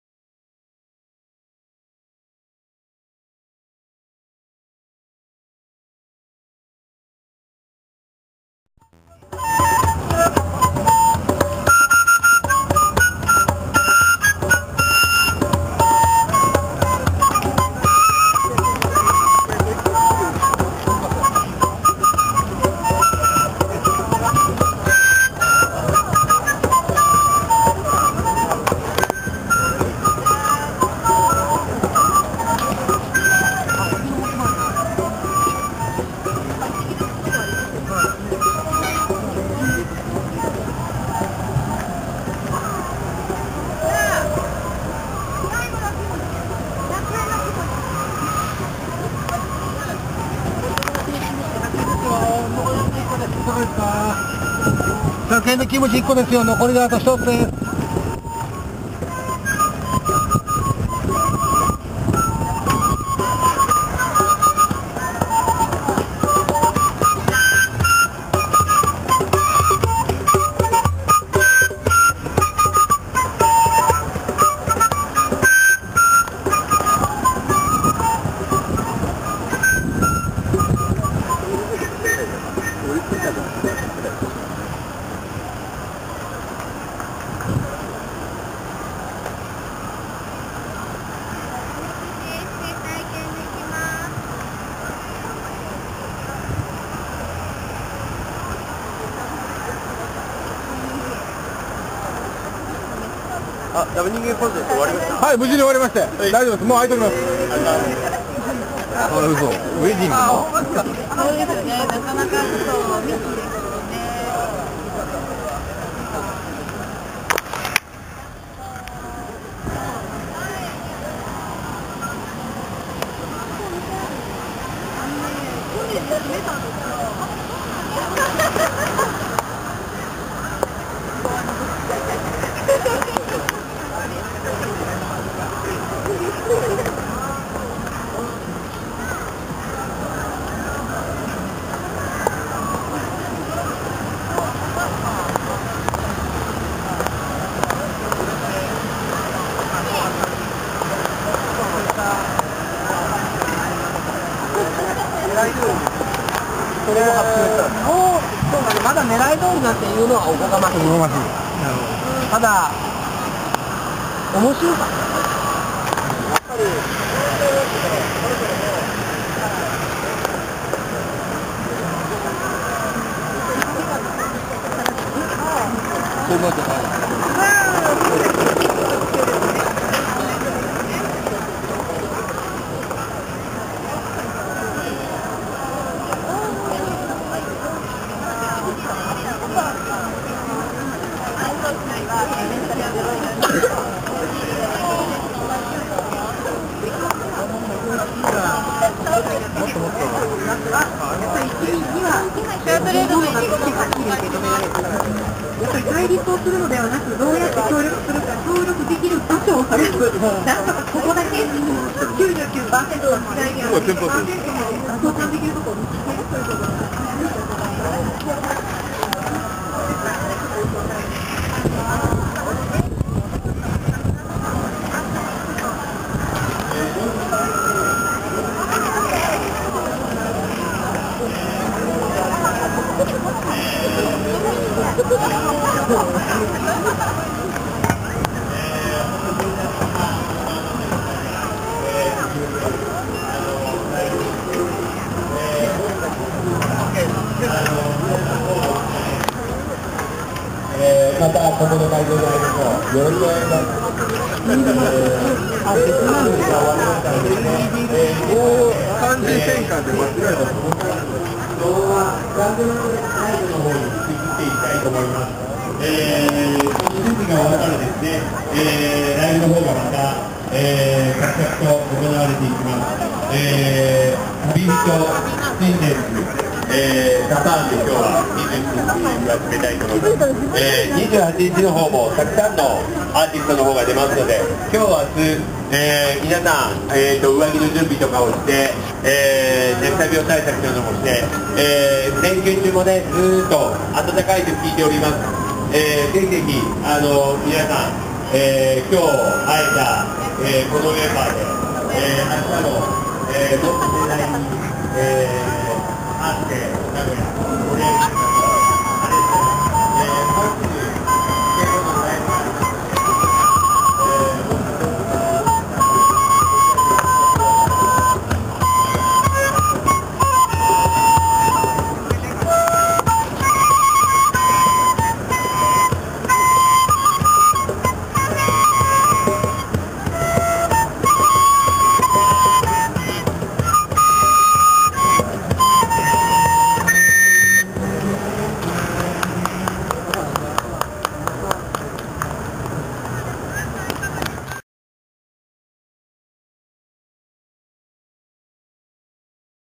アースデイあいち(名古屋)2013・一日目・月ステージ周辺・会場の様子・LOVE&ビンボー春祭り
●アースデイあいち2013・LOVE&ビンボー春祭り

日時: 2013年4月27日(土)28日（日）10時～17時まで。
(28日は17時3半頃より「ちきゅうパレード」あり)
場所: 久屋大通公園・もちのき広場南東端
(2013年のラブビは、アースデイ名古屋アラタワと同じ会場で行います。2008年から別会場で2つ開催されてきた名古屋のアースデイでしたが、2010年から連携を強化した結果、2012年から二年連続で同じ会場で開催されます)

主催:アースデイあいち2013・LOVE&ビンボー春祭り実行委員会
共催:アースデイ名古屋2013・around the tower実行委員会
後援:愛知県教育委員会・名古屋市教育委員会
協賛:第84回愛知中央メーデー

「LOVE&ビンボー春祭り」は、
政治的な課題も誤魔化さずに扱い、行動する、ポリティカル・アースデイであると同時に、
地球で働く寄る辺なき者達の祭典、全国インディーズ系メーデー中部としても開催されています。